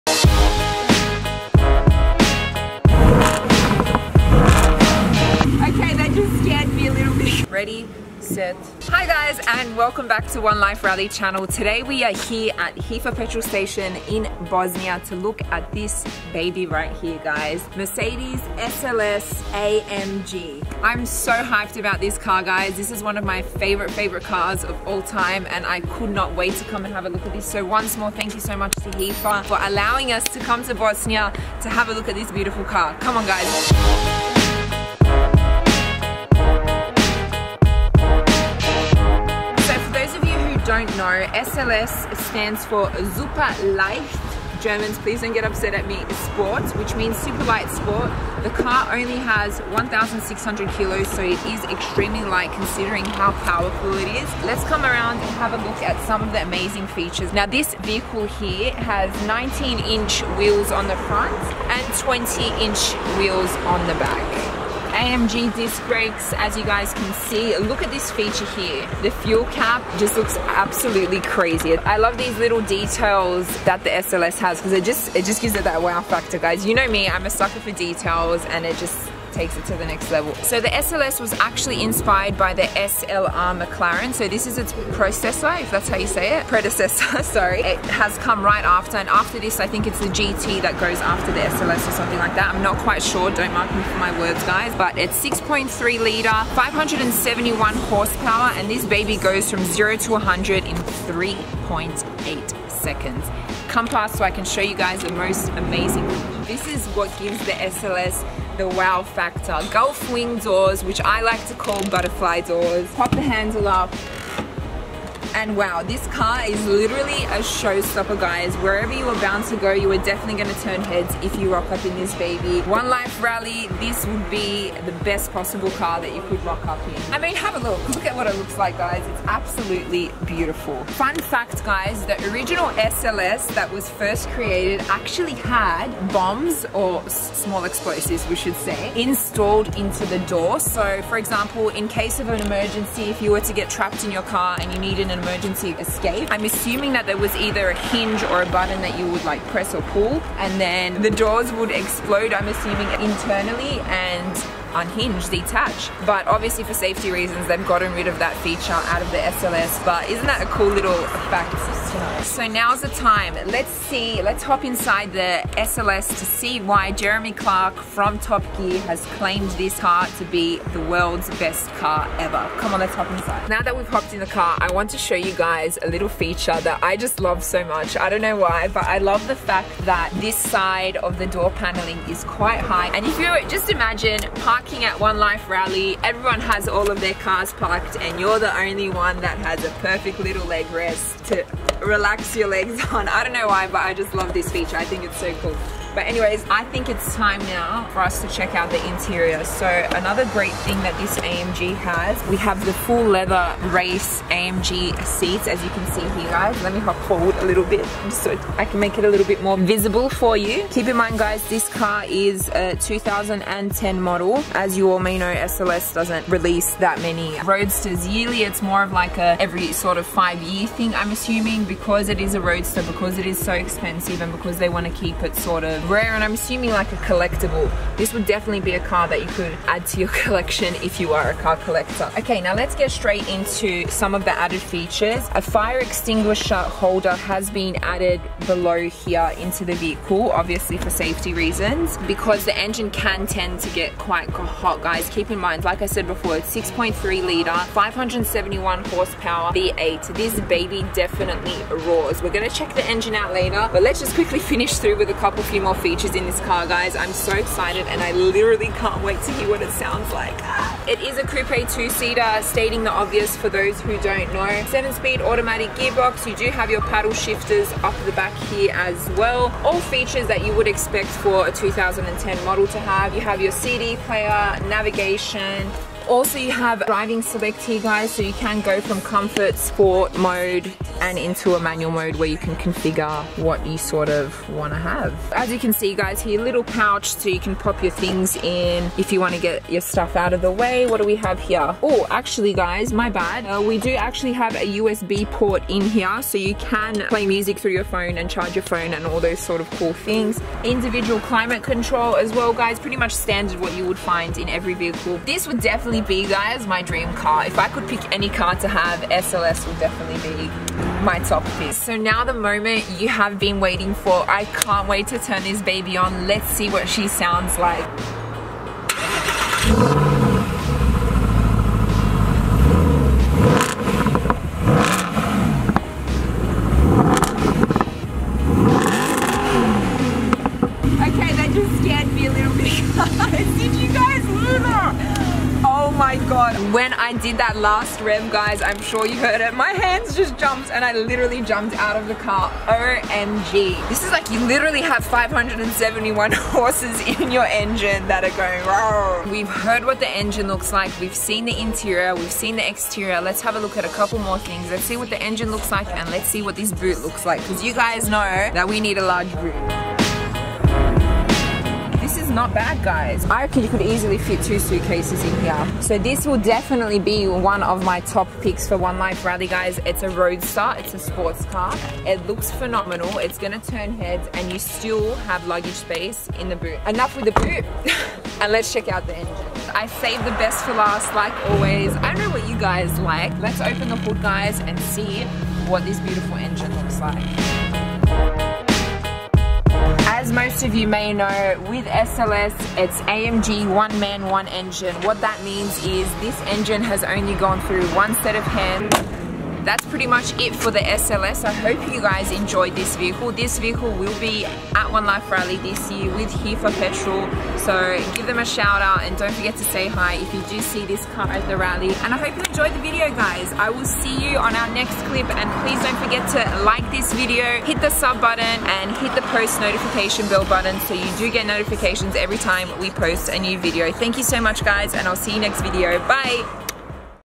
The $185,000 Butterfly Door SUPERCAR | Mercedes SLS AMG In-depth Review
Welcome to LAURA'S LIST - where every Friday, Laura reviews a new supercar!

Copyright • OneLifeRally 2020